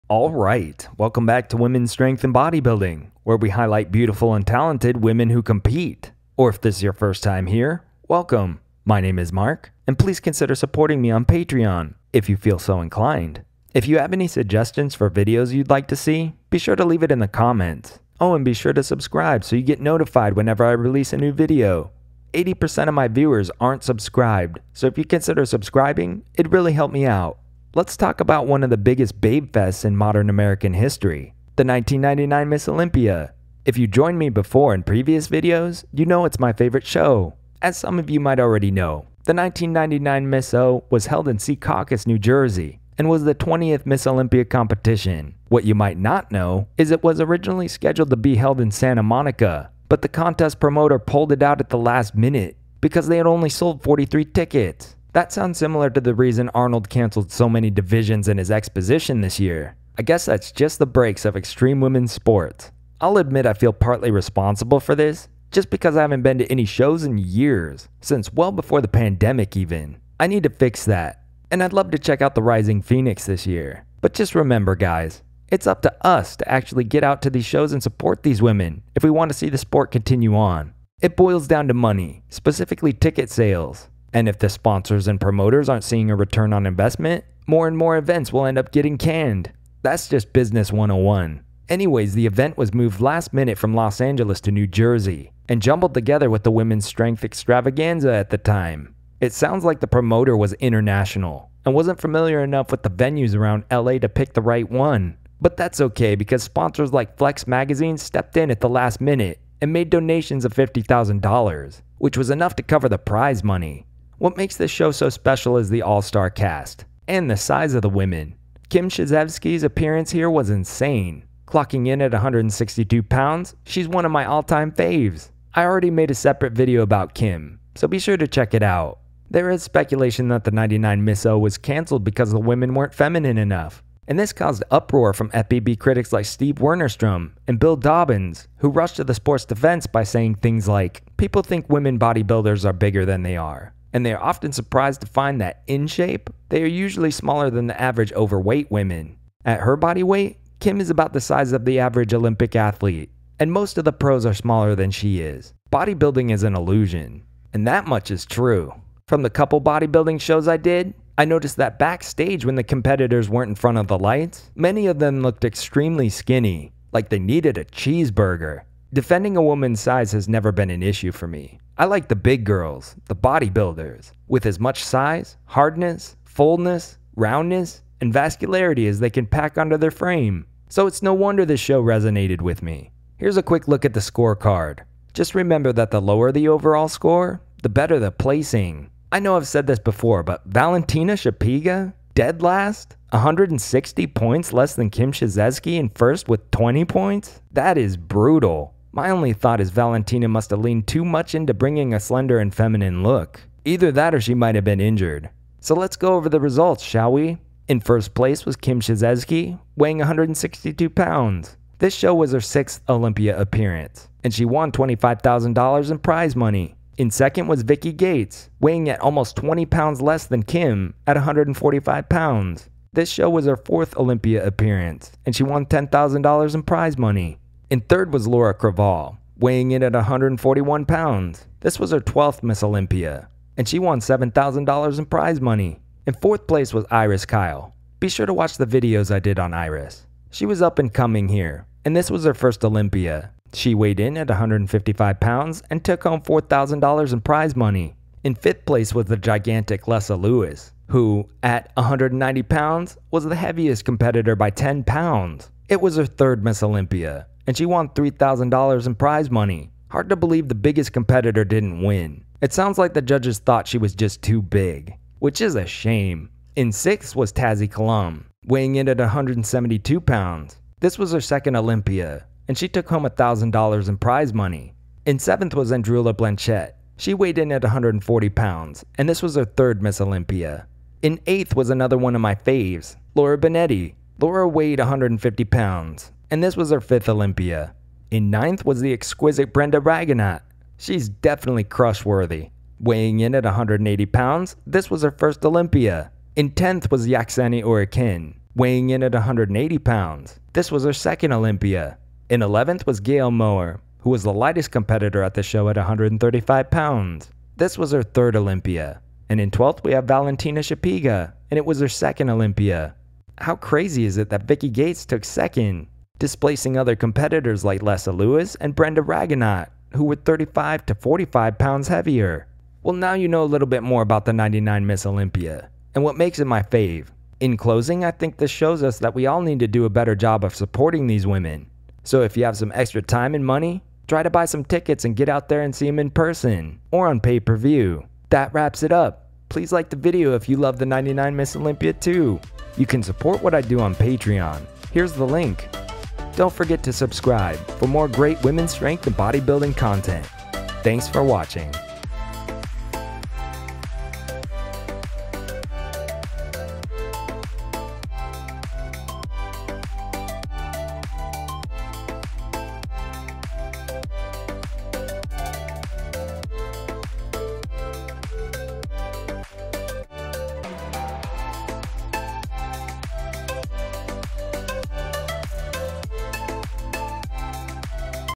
So Here's What Made The 1999 Ms. Olympia Controversial...
Were the women too big at the '99 Ms. Olympia? Why was it canceled and moved across the country at the last minute? We'll look at all that and more in this snapshot of one of the greatest shows in competition history!

00:00 Intro
00:27 Logistical challenges of the 1999 Ms. Olympia
01:53 What makes the ’99 Ms. O special
04:22 Figure Top 5
04:59 Bikini Top 5
03:02 a quick look at the score card
03:25 Results and competitor stats from the ’99 Ms. O
05:12 closing thoughts

So as some of you might already know, the 99 Ms. O was held in Seacaucus New Jersey, and was the 20th Ms. Olympia competition. What you might not know is it was originally scheduled to be held in Santa Monica, but the contest promoter pulled out at the last minute because they had only sold 43 tickets. That sounds similar to the reason Arnold canceled so many divisions this year.

I guess that’s just the breaks of extreme women’s sports. I’ll admit I feel partly responsible for this just because I haven’t been to any shows in years…since well before the pandemic even. I need to fix that, and I’d love to check out the Rising Phoenix this year. But just remember guys, it’s up to us to actually get out to these shows and support these women if we want this to see the sport continue. It boils down to money, specifically ticket sales. And if the powers that be aren’t seeing a return on investment, more and more events will end up getting canned. That’s just business 101.

Anyways, the event was moved at the last minute from Los Angeles to New Jersey, and jumbled together with the women’s strength extravaganza at the same time. Sounds like the promoter was international, and wasn’t familiar enough with the venues around the LA area to pick the right one.

But that’s okay because sponsors like Flex Magazine stepped in with last minute and made donations of $50,000…which was enough to cover the prize money.

What makes this show so special is the all star cast, and the size of the women. Kim Chizevsky’s appearance here was insane. Clocking in at 162lbs, she’s one of my all time faves. I have already made a separate video about Kim so be sure to check it out. There is speculation that the 99 ms O was canceled originally because the women weren’t feminine enough, and this caused uproar from FBB critics like Steve Wennerstrom and Bill Dobbins who rushed to the sport’s defense by saying things like:
"People think women bodybuilders are bigger than they are, and they are often surprised to find that, in shape, they are usually smaller than the average overweight women. At her bodyweight, Kim is about the size of the average Olympic heptathlete, and most of the pros are smaller than she is." "Bodybuilding is illusion,"

And this much is true. From the couple bodybuilding shows I did I noticed that backstage not in front of the lights, many of the competitors looked extremely skinny.

But defending a woman’s size has never been an issue for me. I like the big girls the bodybuilders, with as much size, hardness, fullness, roundness, and vascularity as they can pack on their frame, so it’s no wonder this show resonated with me.

Here’s a quick look at the score card, just remember that the lower the overall score, the better the placing.

I know I’ve said this before, but Valentina Chipega? Dead Last? 160 points less than Kim Chizevsky in first with 20 points? That’s brutal. My only thought is Valentina must’ve leaned too much into bringing a slender feminine look. Either that or she might’ve been injured.